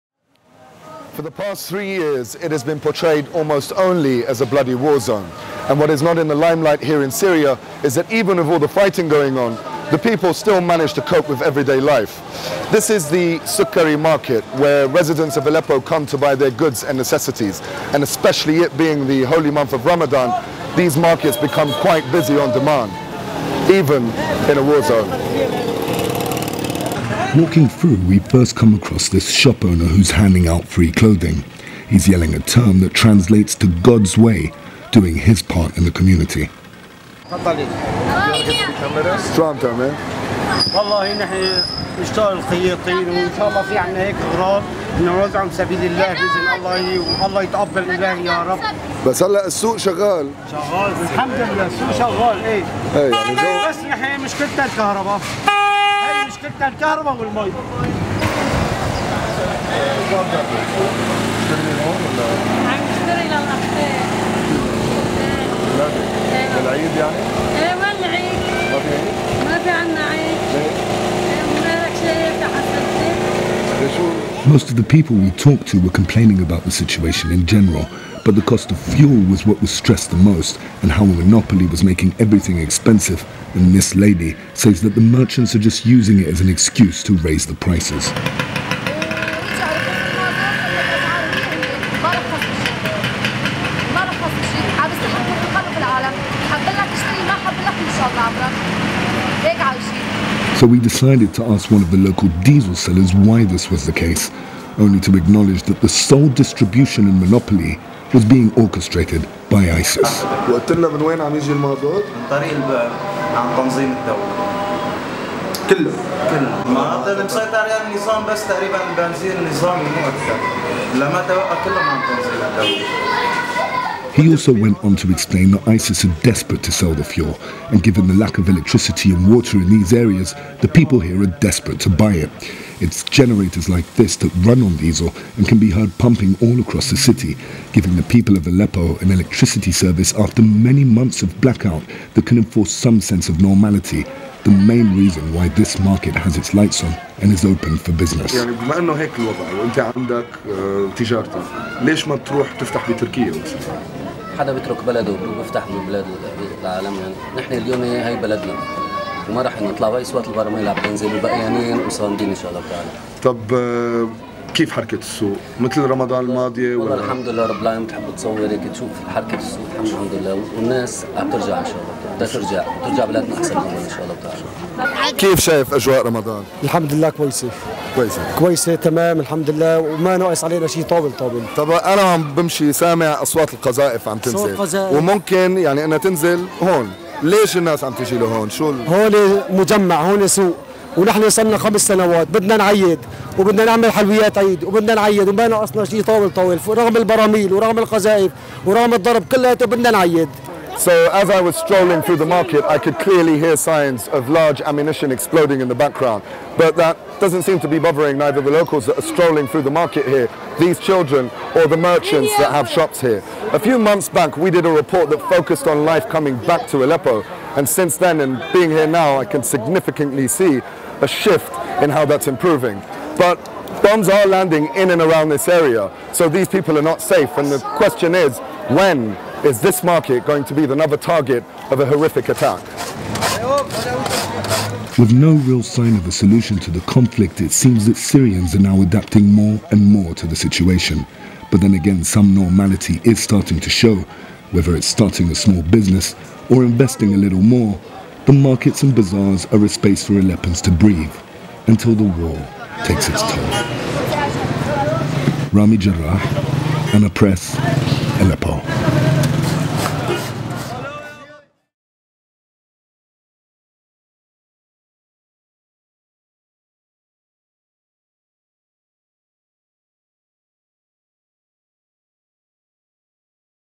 Ramadan in Rebel Controlled Aleppo - Rami Jarrah
Exclusive video report carried out by ANA Press in rebel controlled Aleppo, ANA's Rami Jarrah visits the market places to get an idea on how everyday life looks for Aleppians in the midst of an ongoing war.